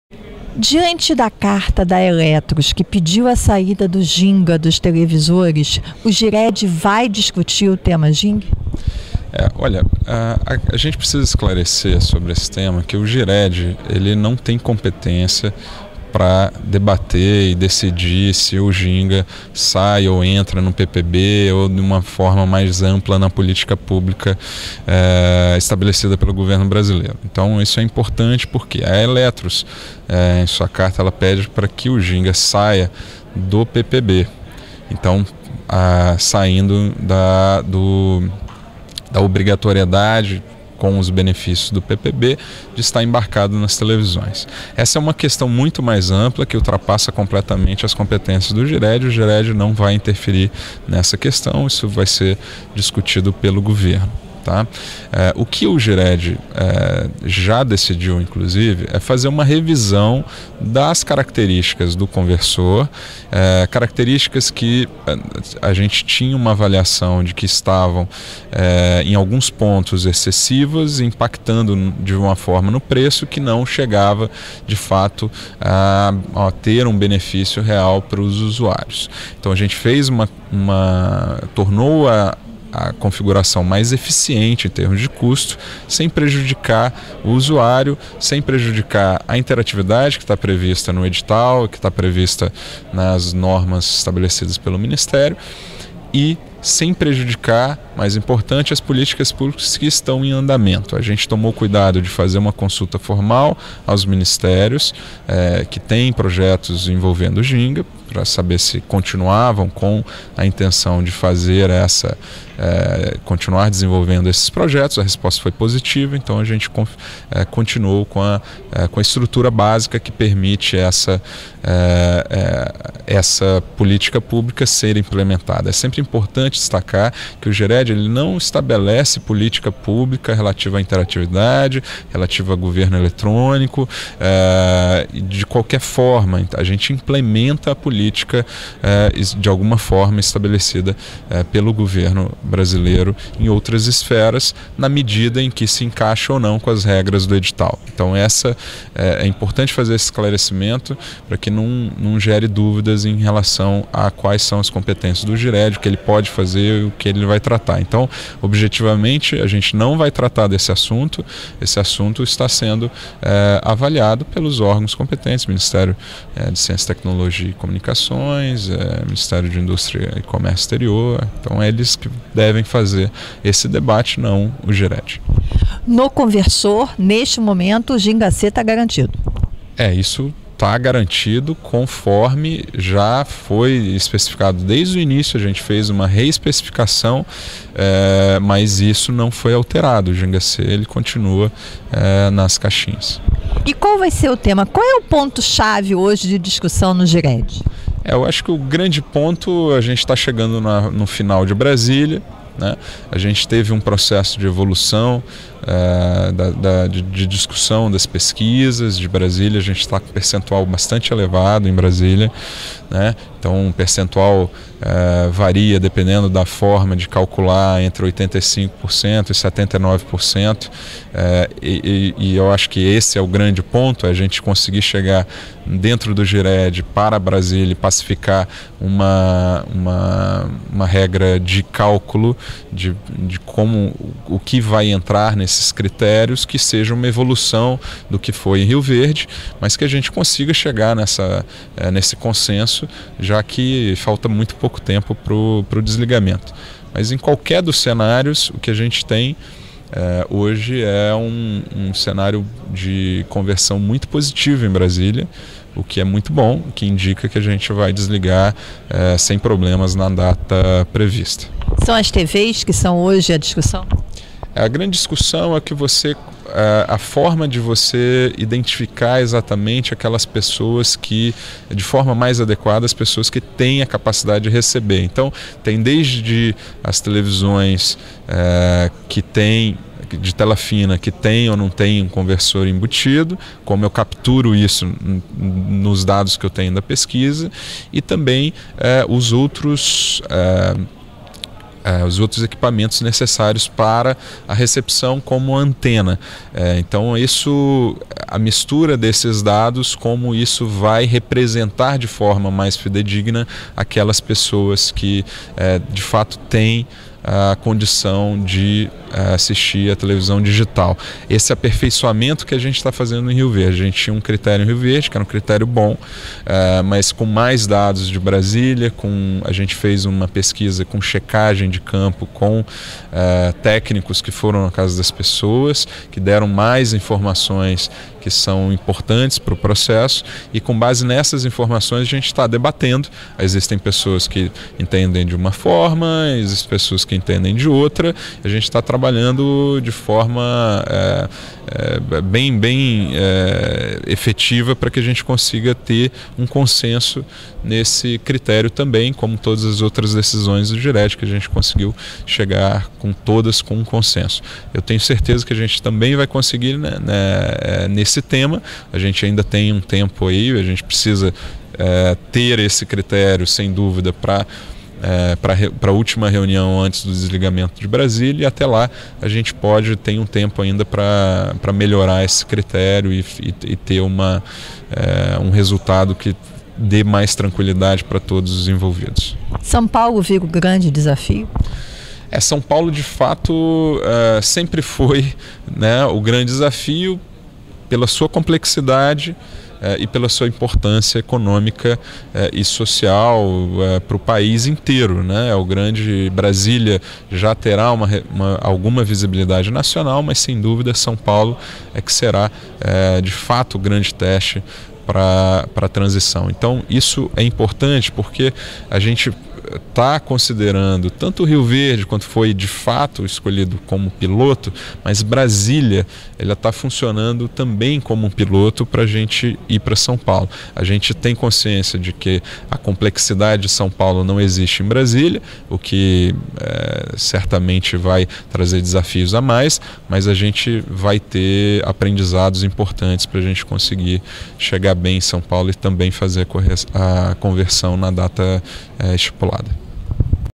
GIRED: Ginga só sai do conversor da TV digital se mudar política pública do Governo
O GIRED, que coordena a migração do serviço analógico de TV para a TV digital, não tem competência para definir política pública e não vai debater o Ginga, além do que já foi discutido - a inclusão do software de interatividade no conversor, com a revisão feita para minimizar os custos de produção, revela o coordenador do GIRED e conselheiro da Anatel, Rodrigo Zerbone, em entrevista à CDTV, do portal Convergência Digital, durante a 16ª Rio Wireless, organizada pela Network Eventos, no Rio de Janeiro.

Zerbone explica que o pleito da ELETROS - de tirar o Ginga da exigência do Processo Produtivo Básico na fabricação de TVs - encaminhado ao Governo será analisado pelos órgãos competentes: os ministérios envolvidos. Até lá, fica valendo o que já foi discutido e aprovado - por consenso - com relação aos conversores: o ginga C está embutido como recurso de interatividade.

"Fizemos uma consulta formal aos ministérios que cuidam das políticas públicas relacionadas ao Ginga se eles iriam manter as apostas no software e a resposta foi positiva. Por isso, implementamos o Ginga no conversor, mesmo com a revisão feita para minimizar os custos. É preciso ficar claro que o GIRED não tem competência para decidir se o Ginga sai ou não de um PPB ou, numa forma mais ampla, de decidir política pública no país", reforça Zerbone. O conselheiro da Anatel fala ainda sobre os temas à mesa no GIRED e do processo de migração para a TV Digital. Assistam a entrevista.